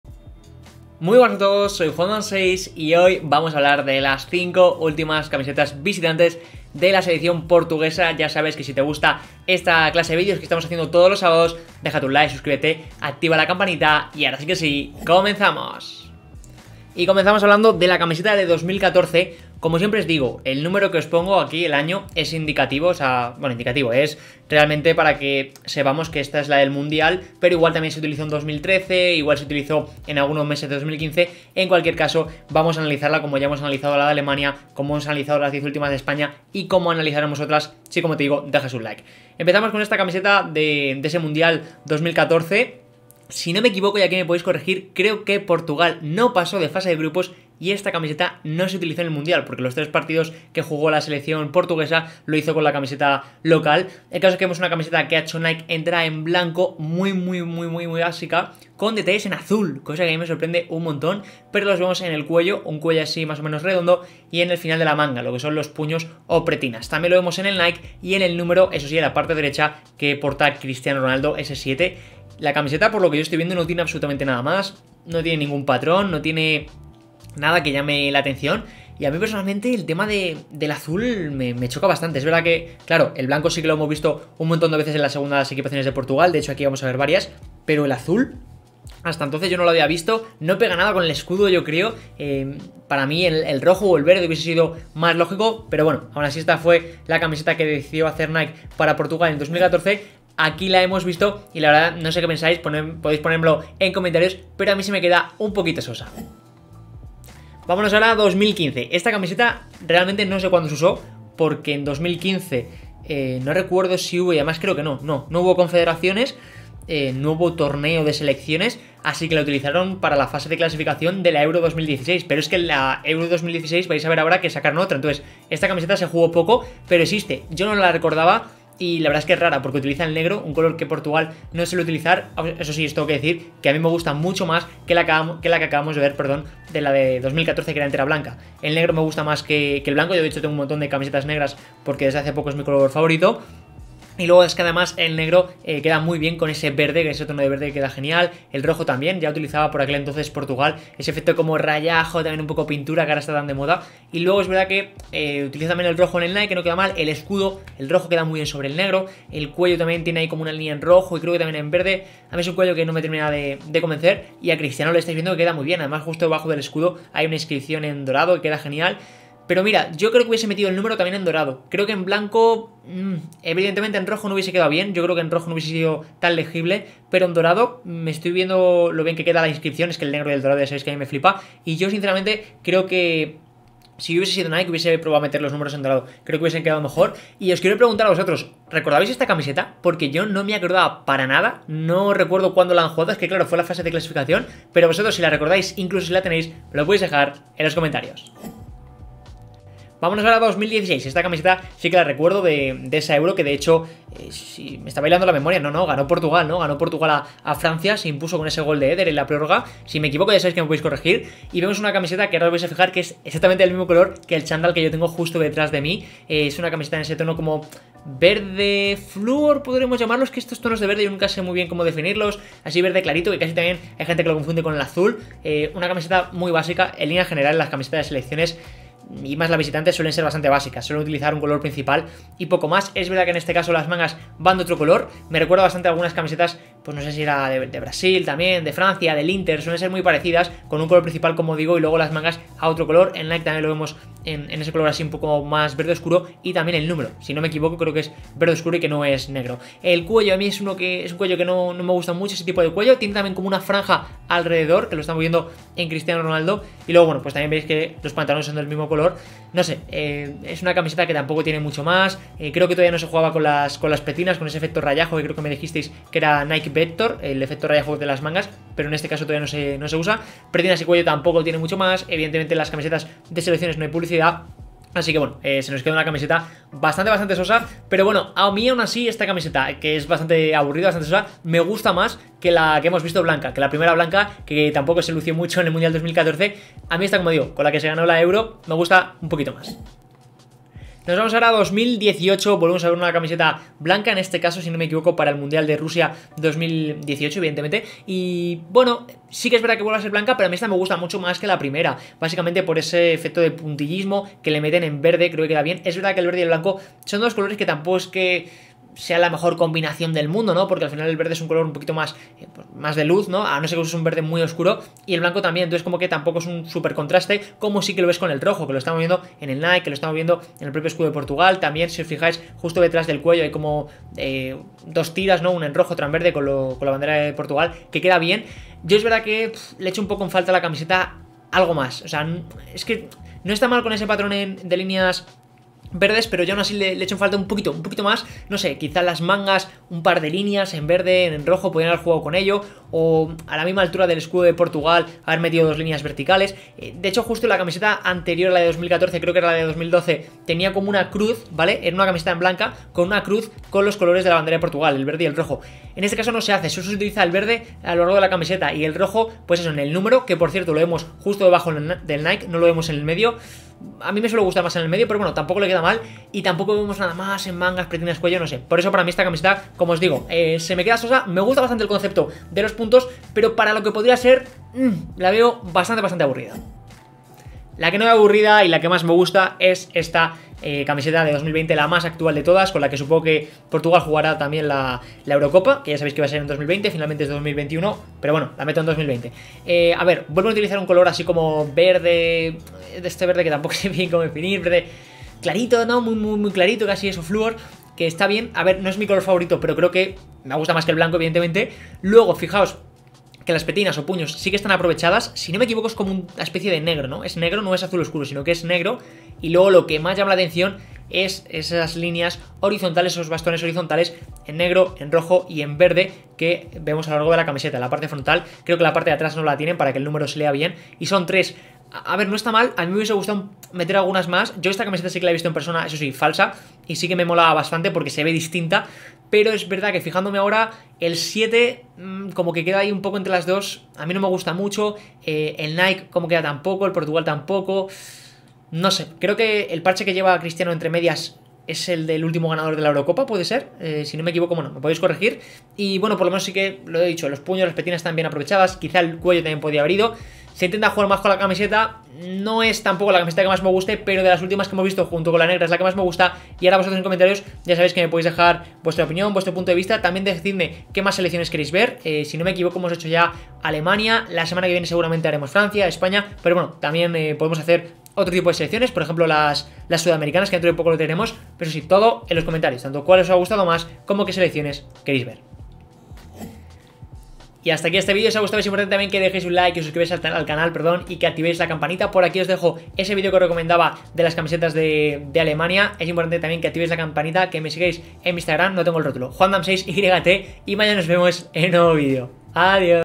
🇵🇹 REACCIONANDO a las 5 ÚLTIMAS CAMISETAS VISITANTES de PORTUGAL
Hoy reacciono a todas las camisetas visitantes de PORTUGAL desde 2014